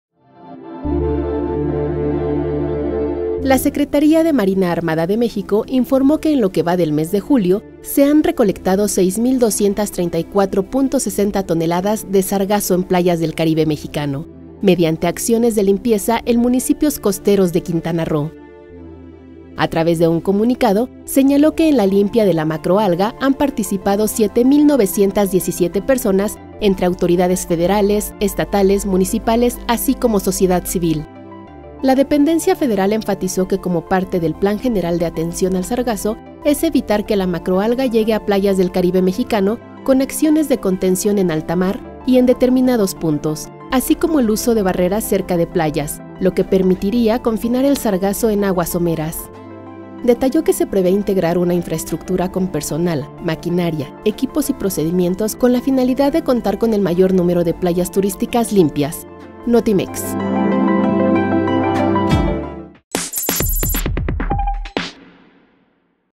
Recolectan más de seis mil toneladas de sargazo en Quintana Roo
En la limpia de la macroalga recolectada han participado siete mil 917 personas, entre autoridades federales, estatales, municipales, así como sociedad civil.